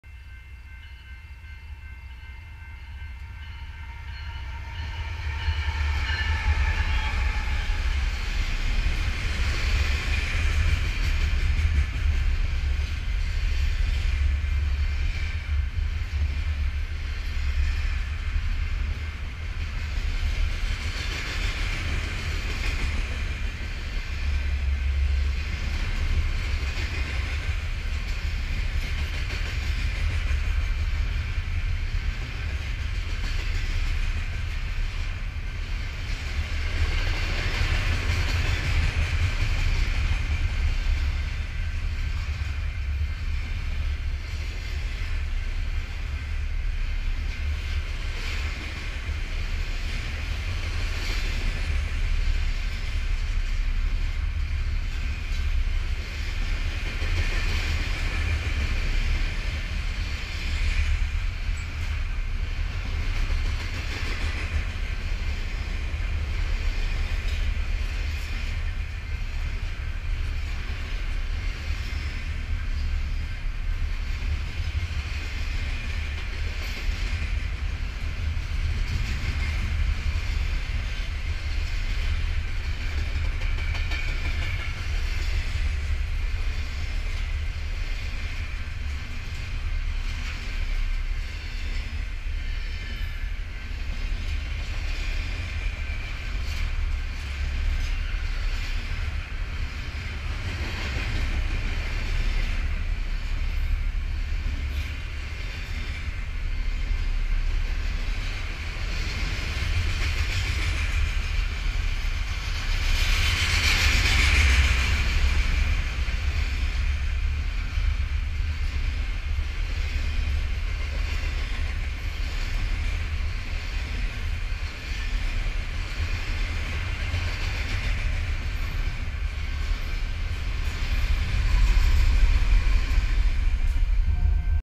EB Intermodal with a Wave Olathe, KS 8-18-15 5:16pm
Videos taken along the Emporia Subdivision in Olathe, KS
"AKA" The Transcon

Video was taken on August 18, 2015 at 5:16 pm near the Santa Fe Street Crossing West of Kansas Avenue on Emporia Sub in Olathe.